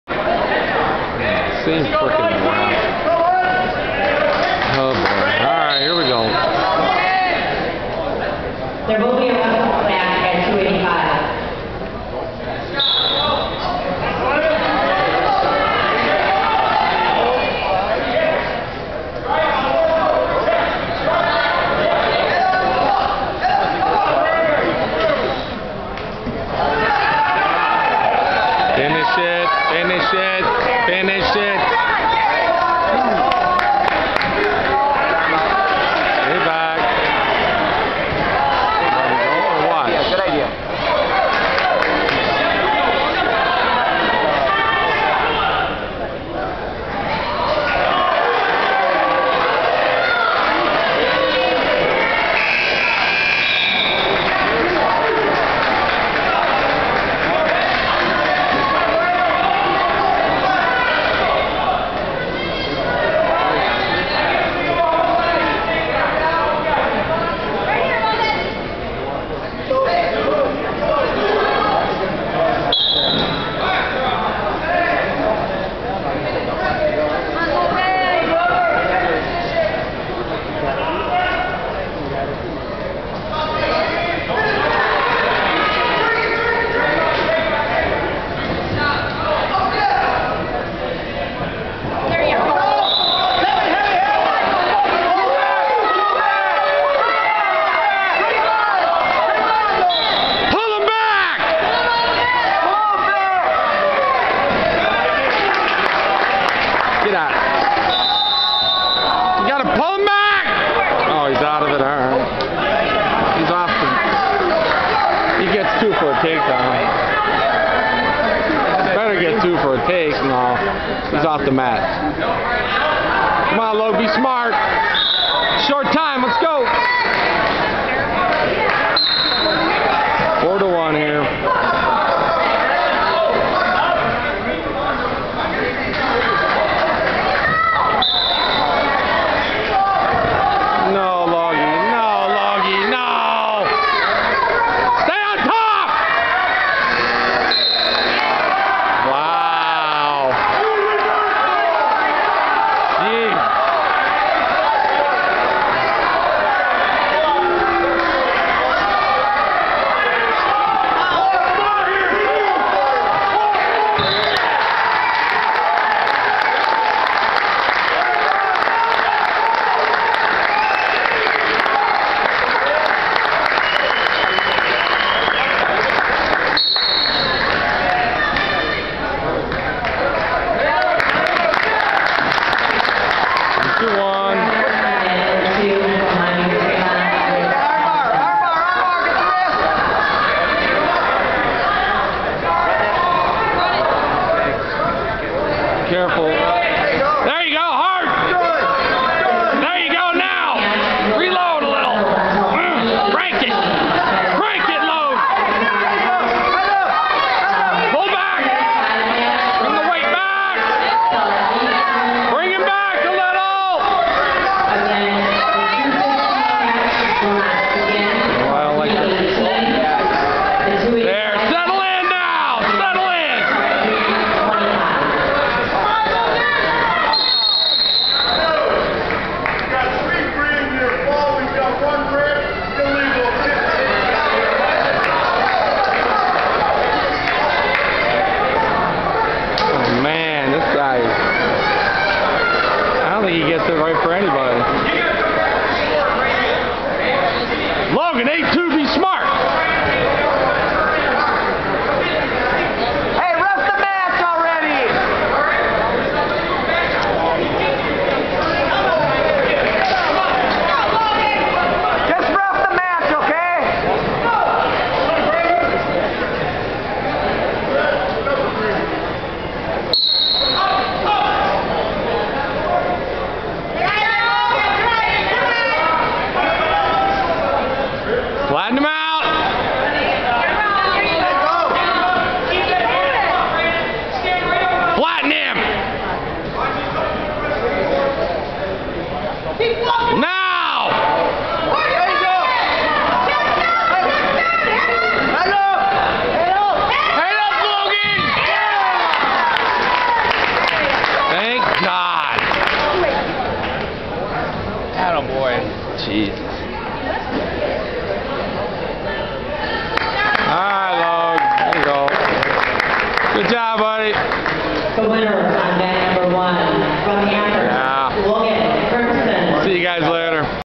Logan Wrestling 035.MOV
Logan is a wrestle back at Regionals. Gets the pin in the second. Again the reffing is suspect like it was all day. I hope these guys don't get any sectional assignments.